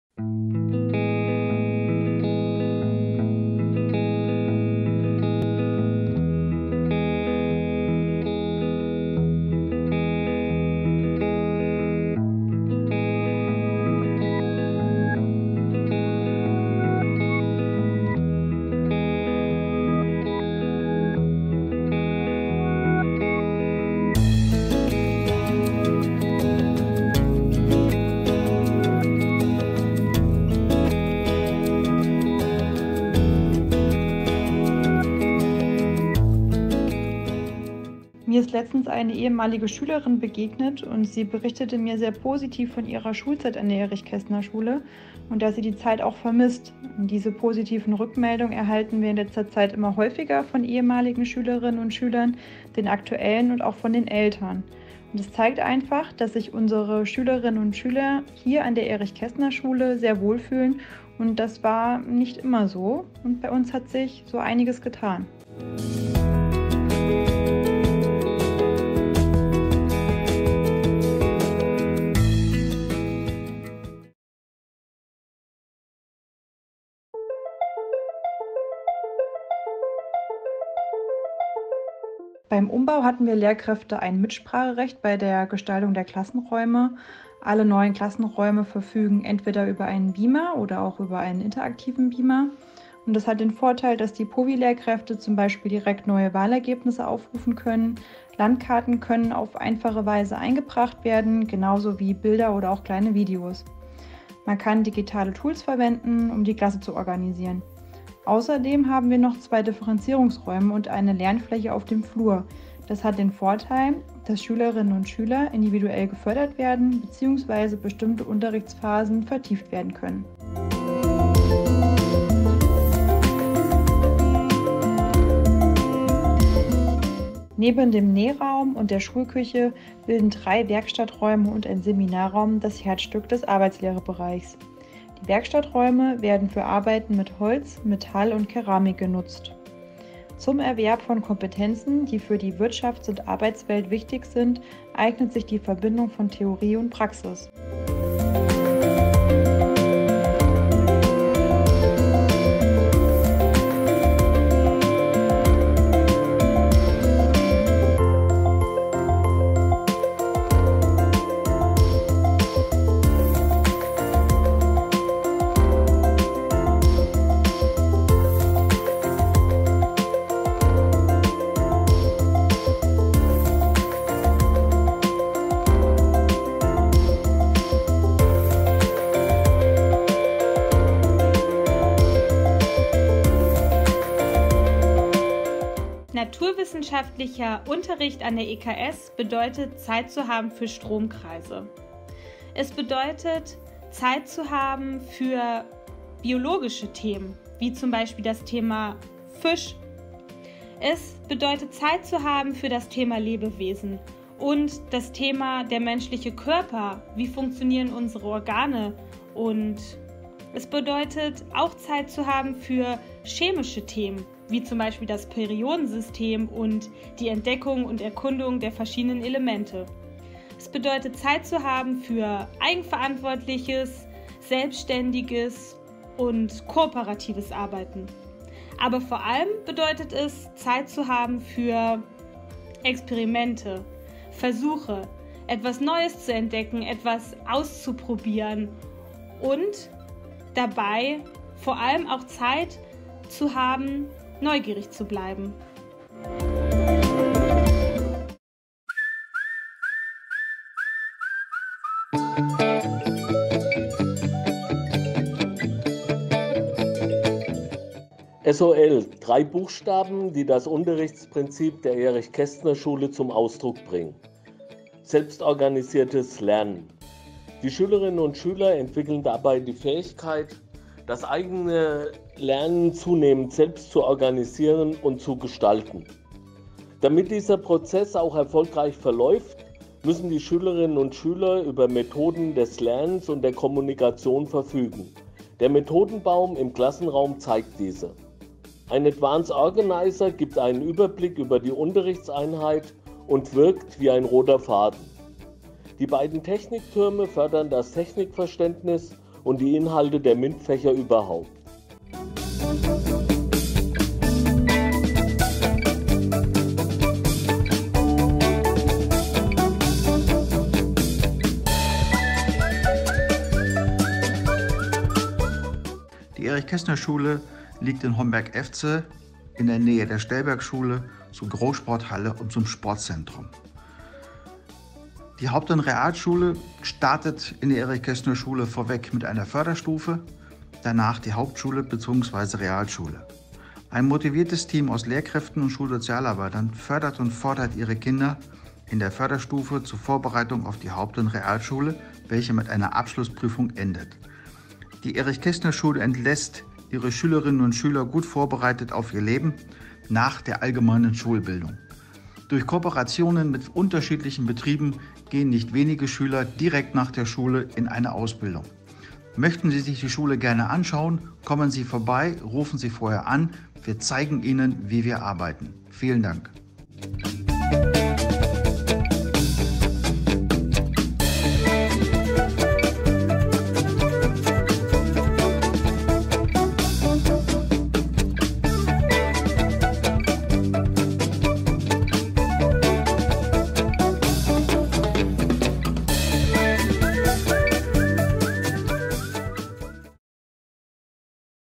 Erich Kästner-Schule Homberg (Efze) Schulvideo
In unserem Schulvideo berichten vier Kolleginnen und Kollegen über die Erich Kästner-Schule in Homberg (Efze).